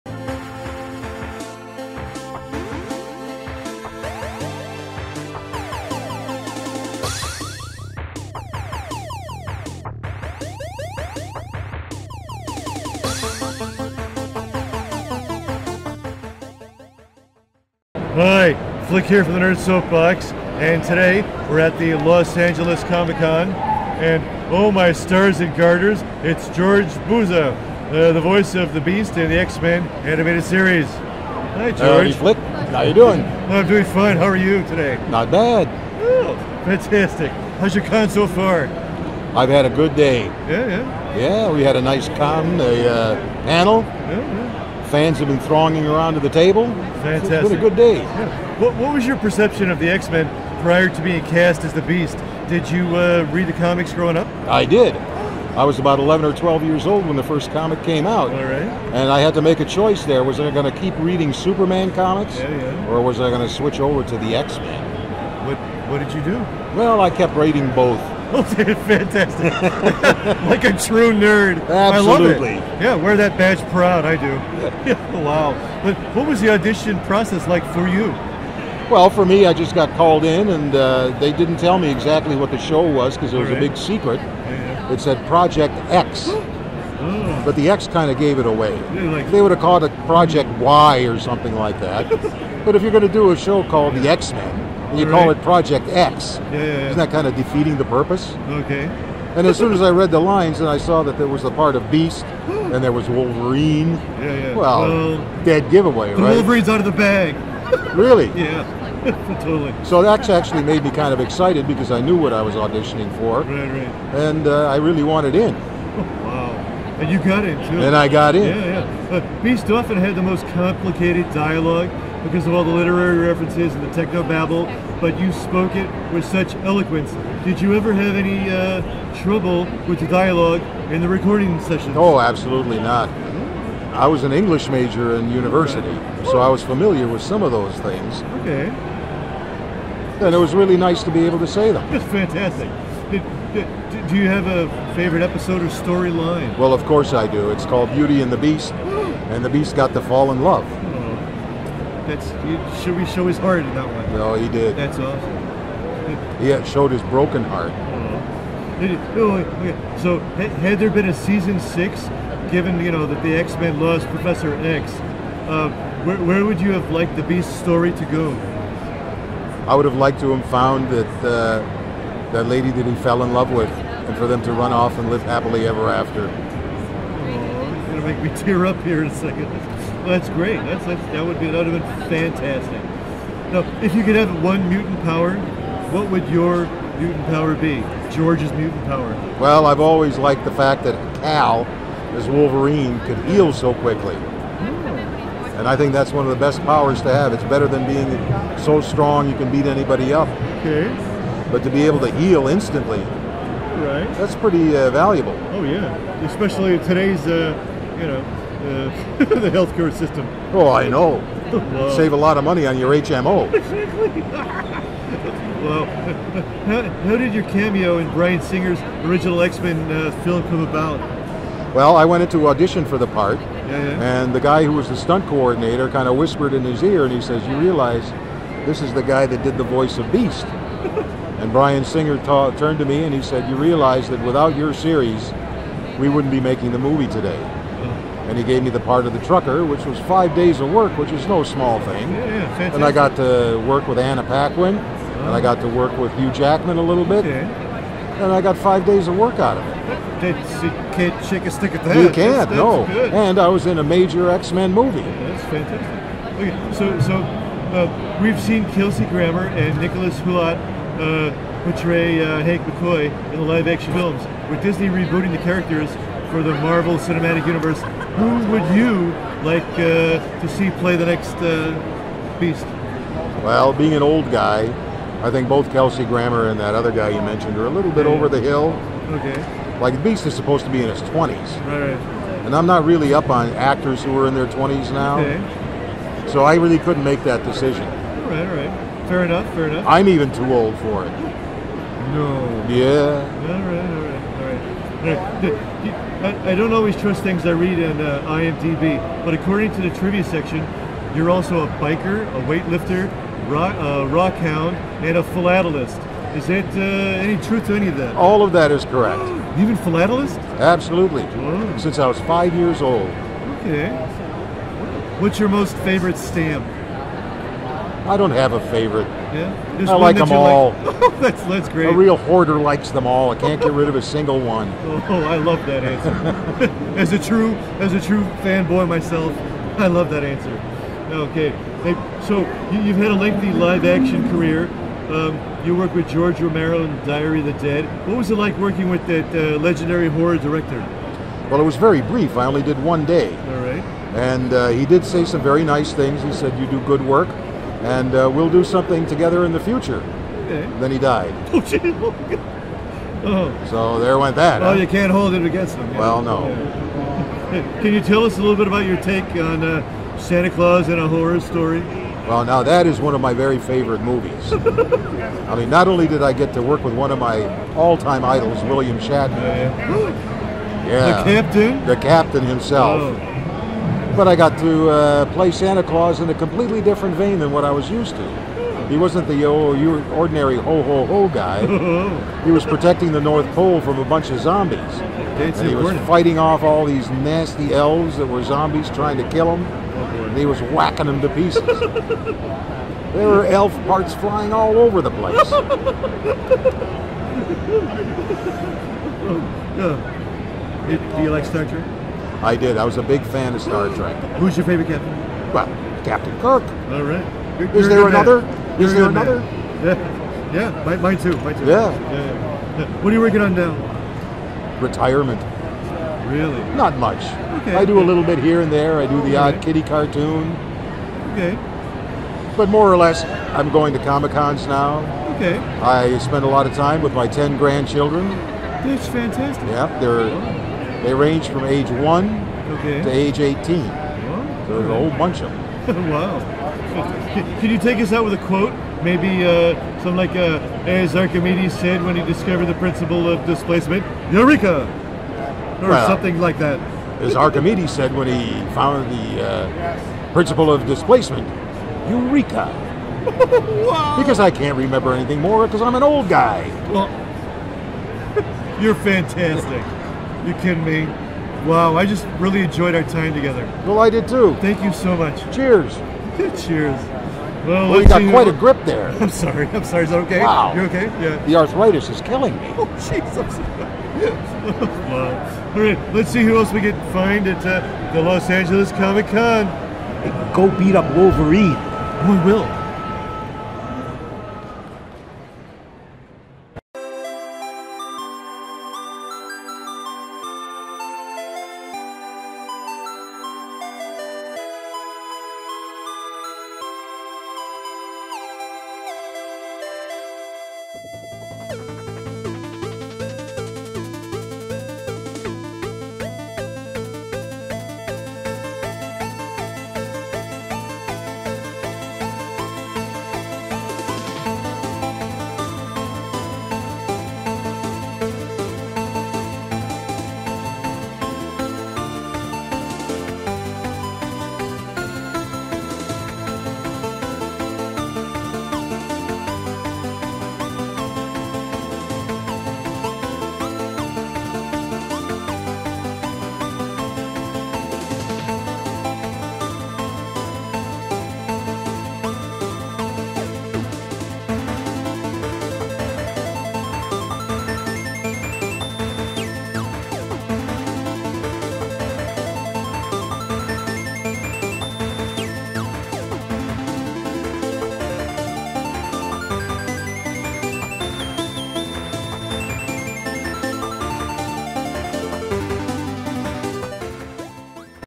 That Time George Buza, Beast from X-Men the animated series, Talked to a Puppet
Hi! Flick here! The 90s X-Men animated series stands as my favorite on-screen adaptation of the X-Men, above all other cartoons and live-action films. That's why it was such a pleasure talking to cast members from the show this past weekend at Los Angeles Comic Con. George Buza, the voice of Beast, sat down with me and we talk about his familiarity with the comics growing up, the mysterious nature of the audition, delivering complicated dialogue, his appearance in the first Bryan Singer X-Men film, working with George Romero on Diary of the Dead, playing Santa Claus in A Christmas Horror Story, and we confirm a series of factoids on his IMDB. "As Archimedes said when he first discovered the Nerd Soapbox, 'Eureka!'"